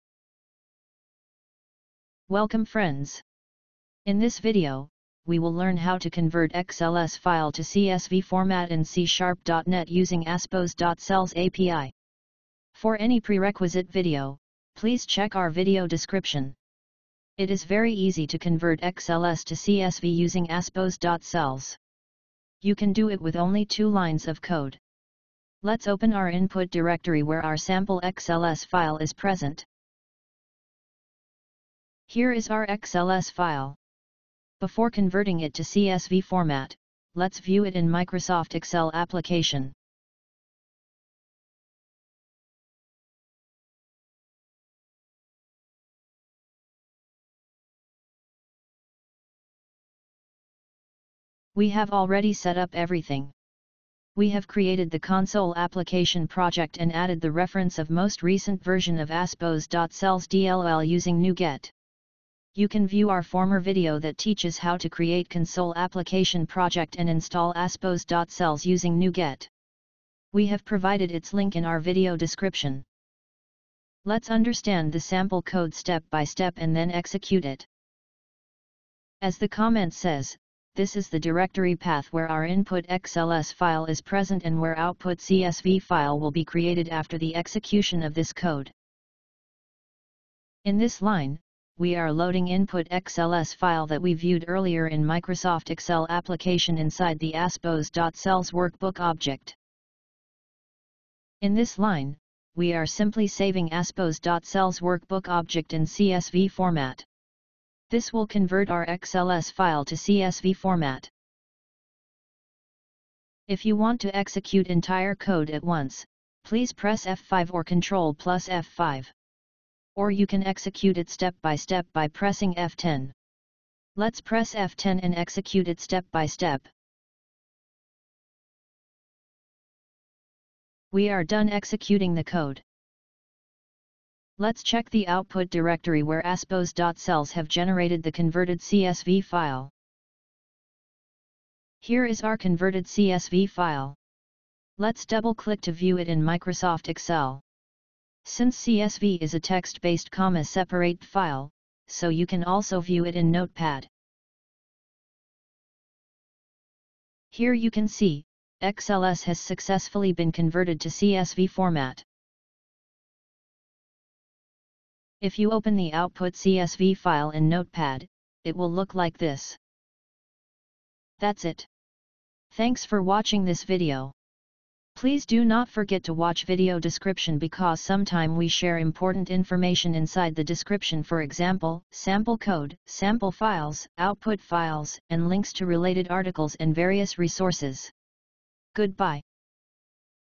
Convert XLS to CSV in C#.NET
Convert XLS to CSV in C#.NET using Aspose.Cells API.

Reference Video - Install Aspose.Cells using NuGet and Create Microsoft Excel Documents in Various Formats

Supported Platforms
Aspose.Cells API supports various platforms including .NET, Java, C++, Android, JavaScript, PHP etc. Besides, Aspose.Cells is available in Cloud as REST or RESTful APIs.

Note:
If you have any comment or feedback, please let us know in comments section.